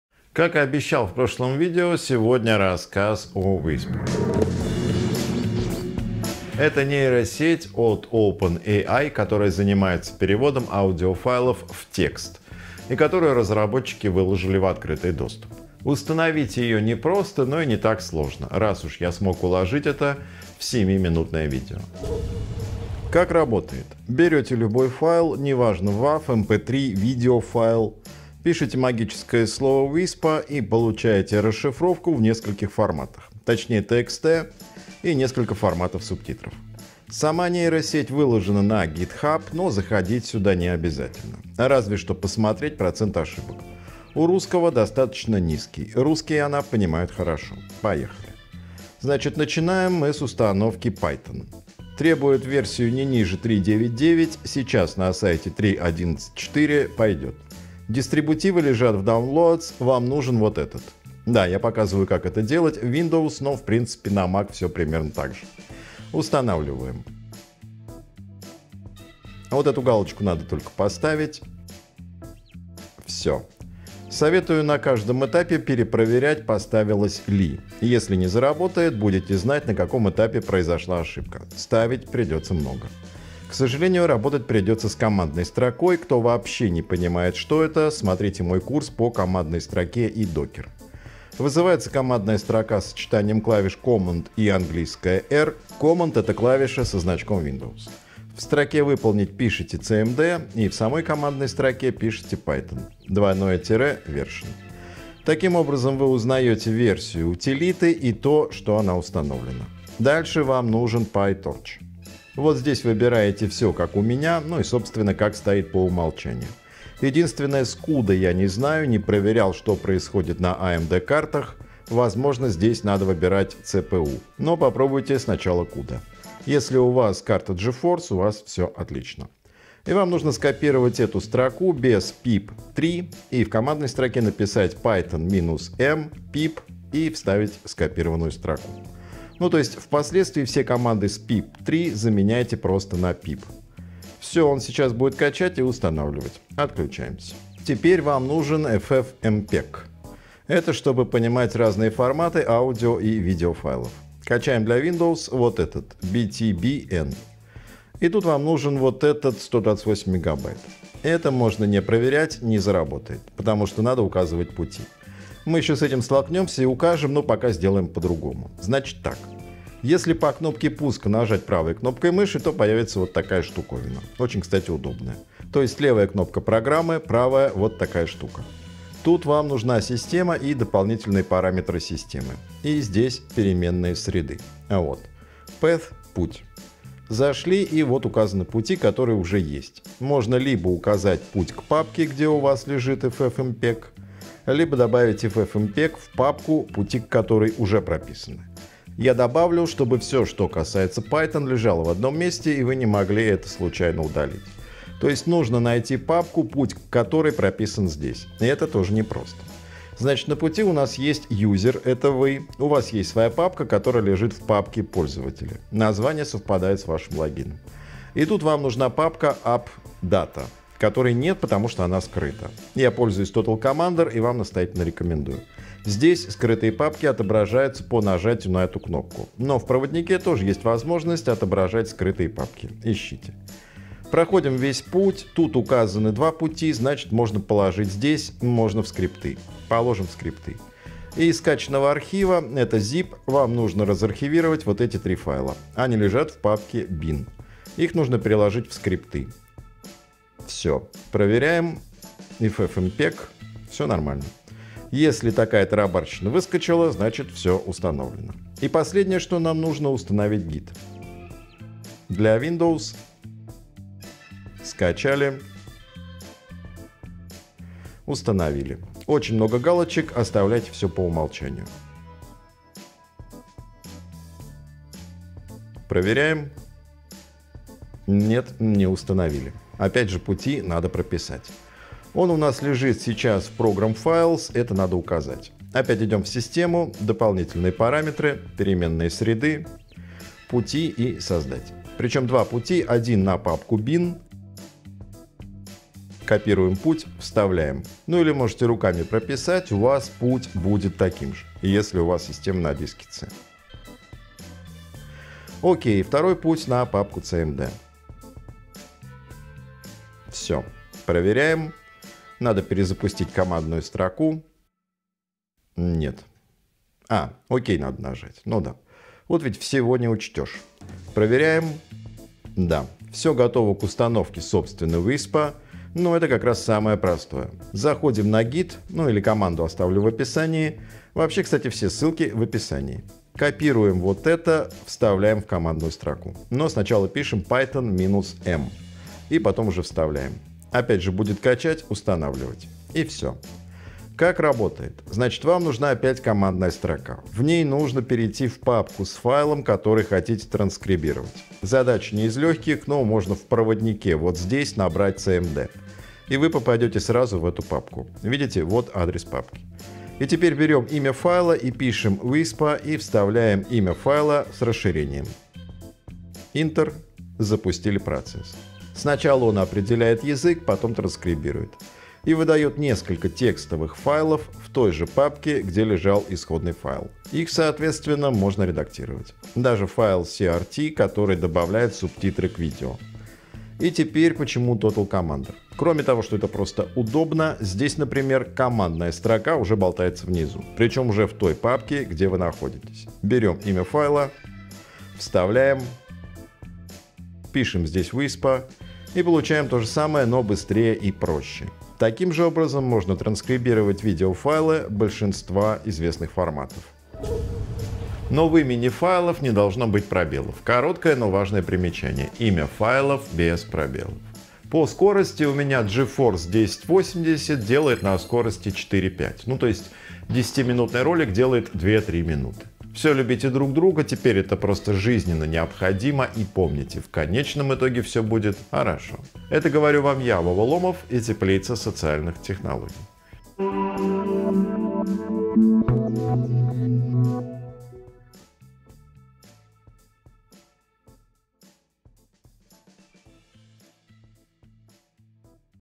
Нейросеть для перевода аудио в текст. Транскрибация от OpenAI: Whisper
OpenAI – передовая лаборатория по разработке нейросетей. Достаточно сказать, что ChatGPT является одним из продуктов OpenAI. Whisper это еще одна нейросеть от OpenAI, которая умеет переводить аудиофайлы в текст.

Делает она это весьма хорошо, но транскрибация – не единственное ее достоинство. Кроме того, что она прекрасно понимает речь на разных языках, она еще и выложена в открытый доступ и любой желающий может скачать Whisper, установить на свой компьютер и пользоваться абсолютно бесплатно и автономно. Рассказываем, как установить Whisper из-под Python.

Таймкоды видео:
00:06 Whisper что это
00:22 Как работает Whisper
00:49 Начало. Установка Python
01:51 Установка PyTorch
02:28 Установка FFmpeg
04:17 Установка Git
04:39 Как прописать пути (Path)
05:22 Установка Whisper